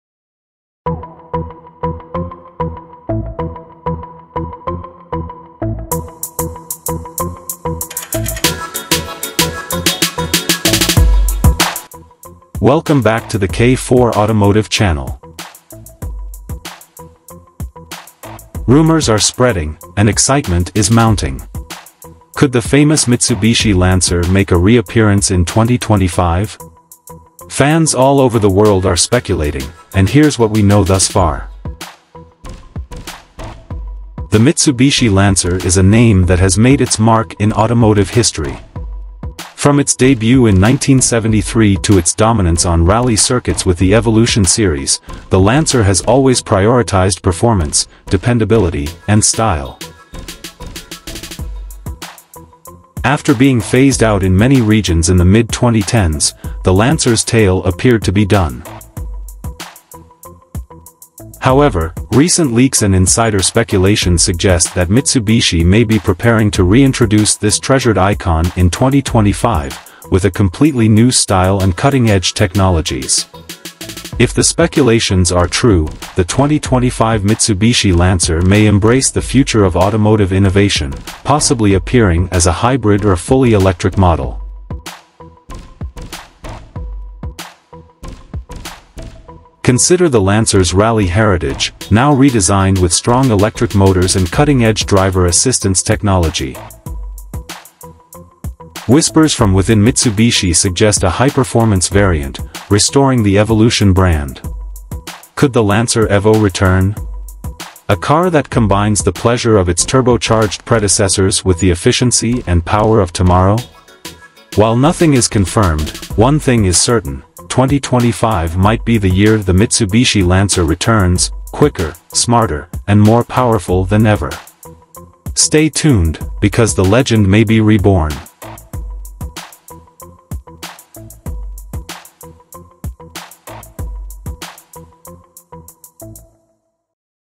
2025 Mitsubishi Lancer Features Revealed – What’s New ?
2025 Mitsubishi Lancer Features Revealed – What’s New ?

Rumors are spreading, and excitement is mounting. Could the famous Mitsubishi Lancer make a reappearance in 2025? Fans all over the world are speculating, and here's what we know thus far.

The Mitsubishi Lancer is a name that has made its mark in automotive history. From its debut in 1973 to its dominance on rally circuits with the Evolution series, the Lancer has always prioritized performance, dependability, and style.

After being phased out in many regions in the mid-2010s, the Lancer's tale appeared to be done. However, recent leaks and insider speculations suggest that Mitsubishi may be preparing to reintroduce this treasured icon in 2025, with a completely new style and cutting-edge technologies.